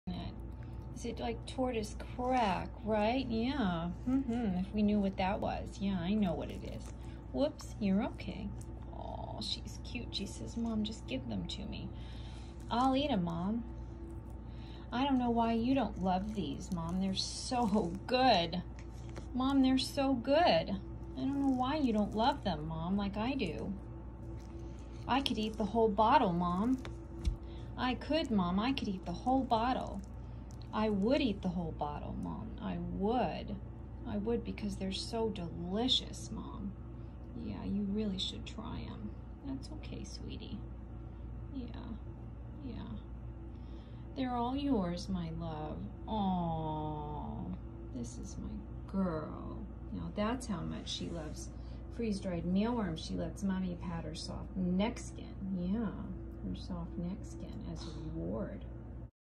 Redfoot tortoise and her favorite snack
Freeze-dried mealworms. Like tortoise crack but legal. 😂🍽🐢